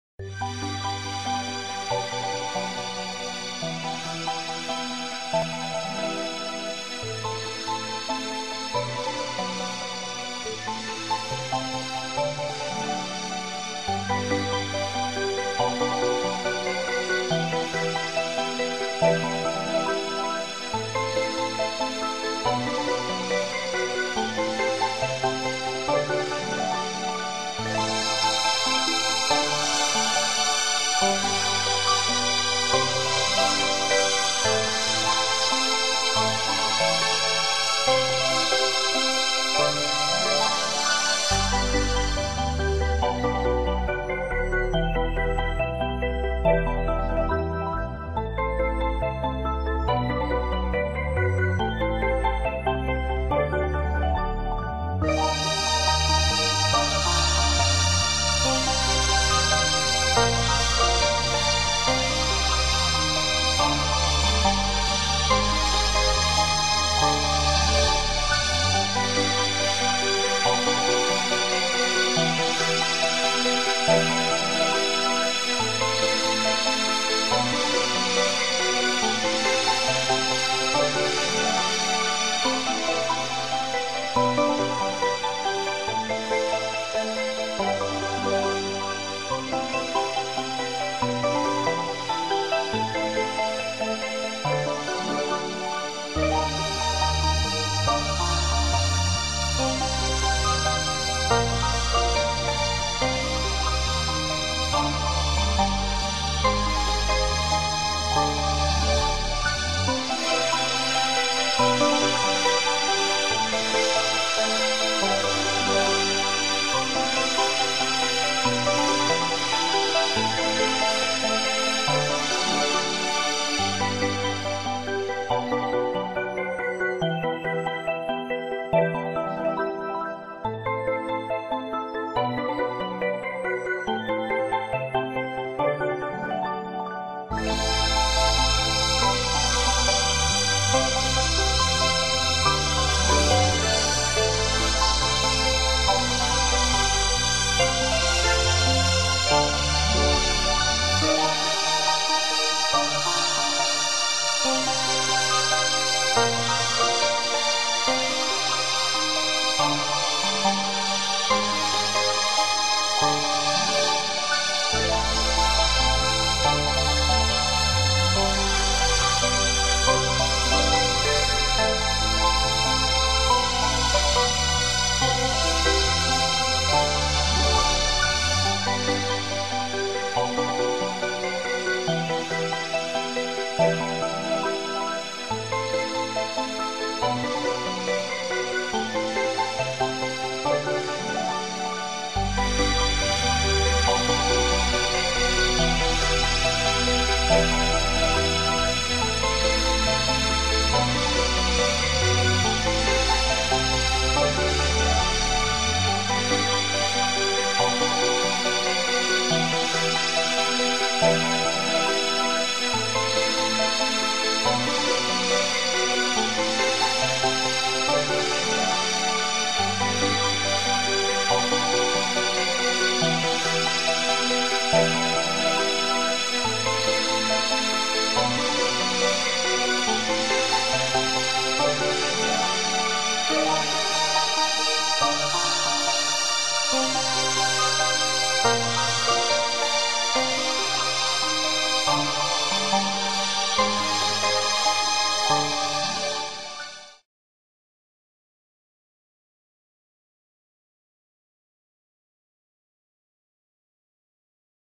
Affirmations for Inner Peace.wmv
Affirmations for Inner Peace is a motivational, inspirational video, with peaceful accompanying music, to help people find their inner peace.